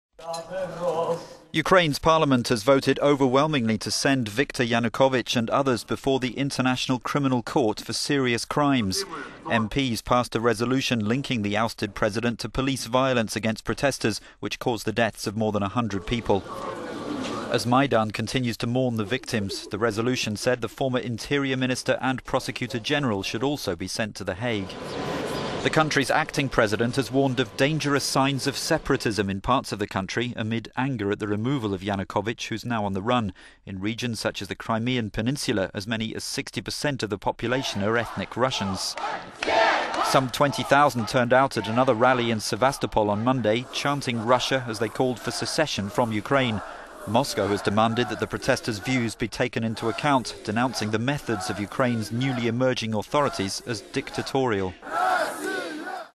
Ukraine leader Turchynov warns of 'danger of separatism'
Ukraine's parliament has voted overwhelmingly to send Viktor Yanukovych and others before the International Criminal Court for "serious crimes".

MPs passed a resolution linking the ousted president to police violence against protesters which caused the deaths of more than 100 people.

As Maidan continues to mourn the victims, the resolution said the former interior minister and prosecutor-general should also be sent to The Hague.

The country's acting president has warned of "dangerous signs of...